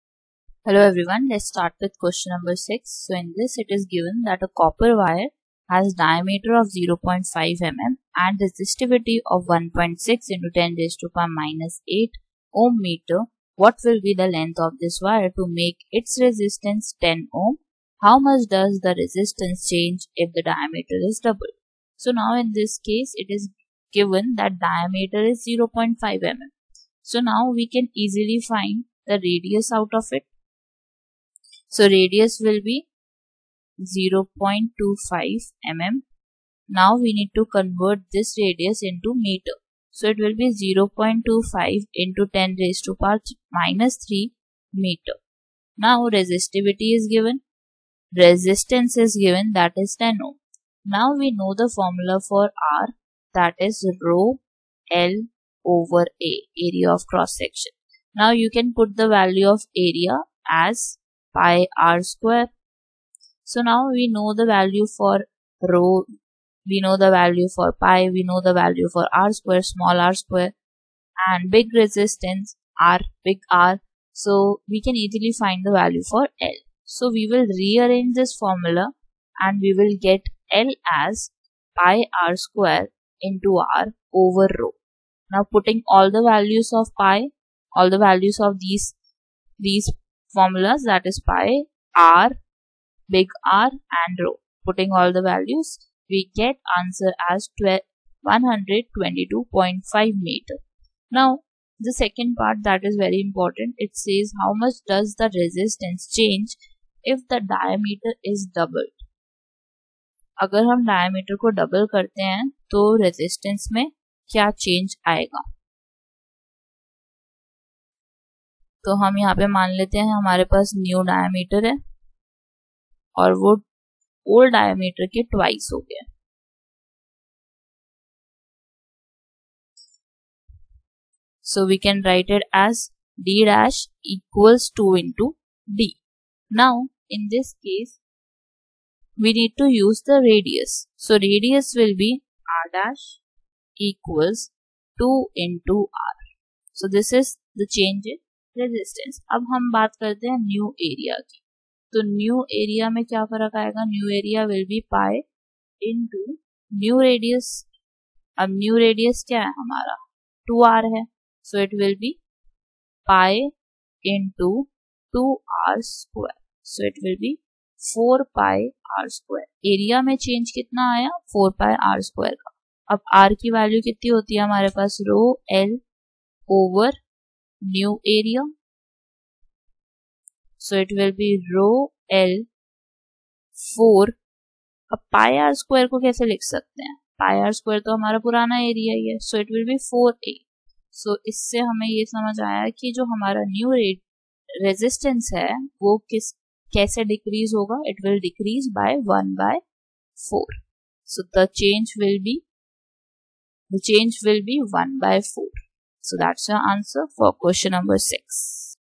Q6 Class X 10th Physics Chapter 12 Electricity NCERT Page Back exercise Solution
6. A copper wire has diameter 0.5 mm and resistivity of 1.6 x 10-8 m. what will be the length of this wire to make its resistance 10? How much does the resistance change if the diameter is doubled?